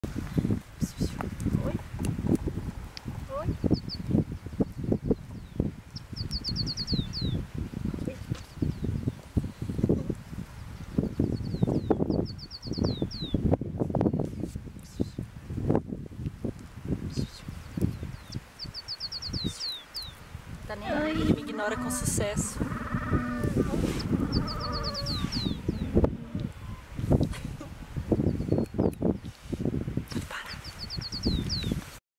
Como ser ignorada com sucesso por um pinguim!
Era pra ser uma filmagem bem romântica, cheia de interação e amor, mas quem disse? O pinguim de Magalhães me ignorou com sucesso, desconsiderando as minhas tentativas de contato pra essa filmagem e aí eu fiquei com vergonha de continuar filmando. Vale a piada! E também vale saber que quando eu não tentava filmar, eles eram bem mais atenciosos comigo! :) somostodospinguins Filmagem feita na Península de Valdes, Chubut, Argentina - Estancia San Lorenzo.